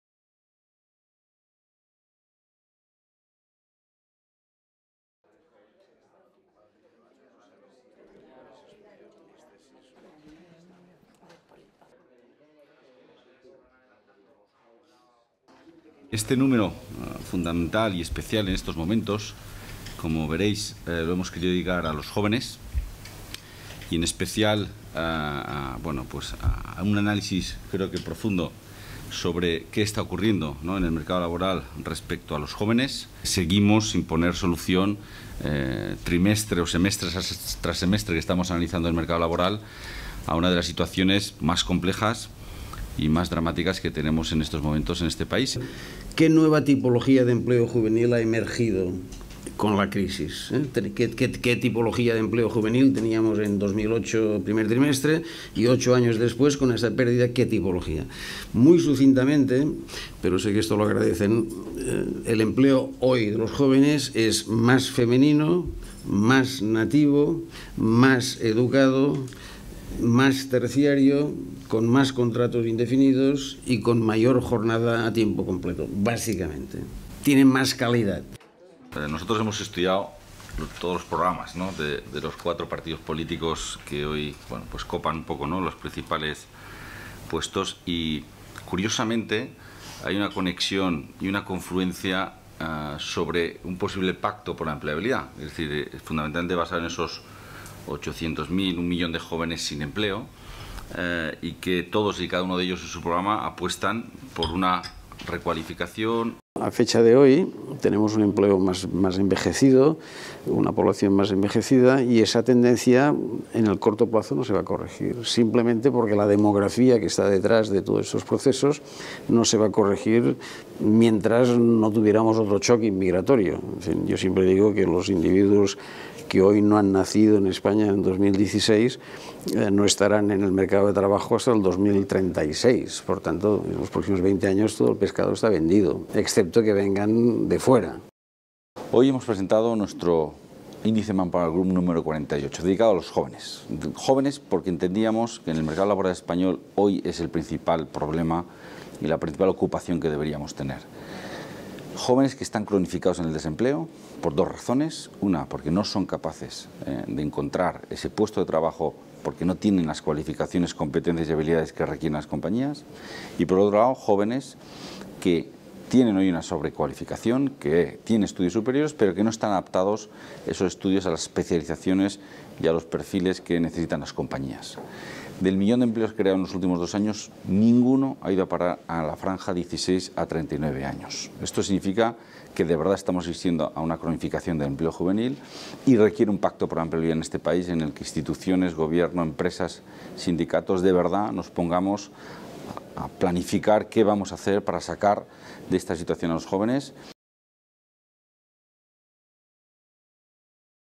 Índice ManpowerGroup: 2008-2016: Retos y paradojas del mercado laboral en los jóvenes en España
Presentación del  "Índice ManpowerGroup: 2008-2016: Retos y paradojas del mercado laboral en los jóvenes en España" a cargo de Raúl Grijalba, presidente ejecutivo de ManpowerGroup en España y por Josep Oliver, catedrático de economía aplicada de la Universidad de Barcelona.

Entre el primer trimestre de 2008 y el primero de 2016 han desaparecido 3,6 millones de empleos juveniles. El impacto demográfico ha acabado con 1,9 millones de puestos de trabajo y la menor demanda de las empresas ha supuesto 1,7 millones de empleos perdidos, a lo que se ha unido una reducción en la tasa de actividad de los jóvenes (por menos actividad, vuelta a estudios, etc.).